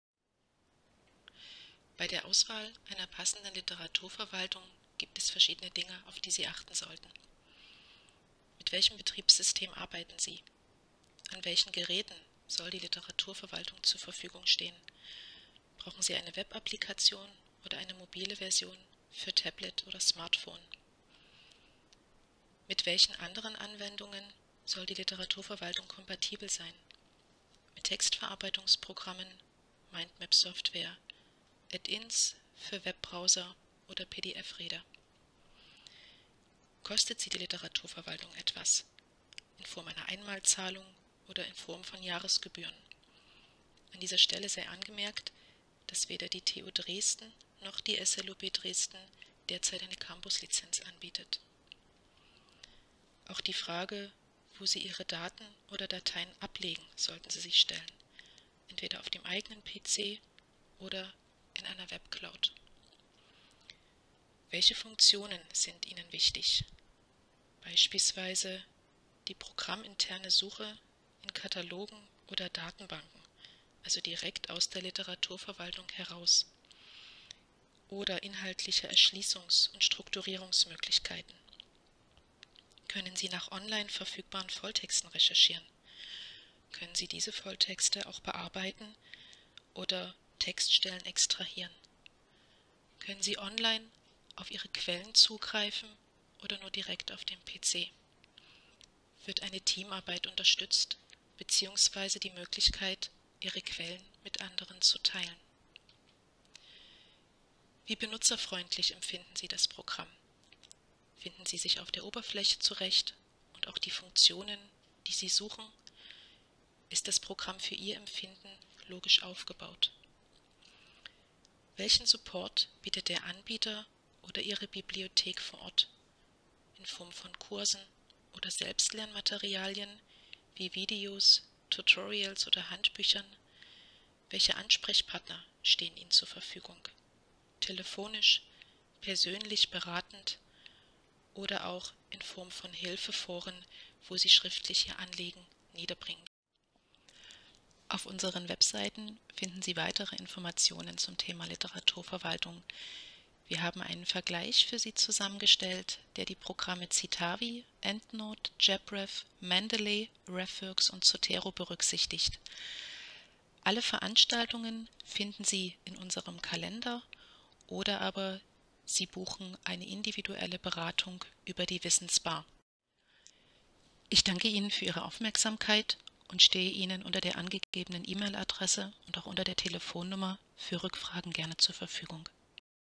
SLUBcast - Literaturverwaltung im Überblick Teil 4
Vierter Teil des Kurses Literaturverwaltung im Überblick - Programmauswahl und Support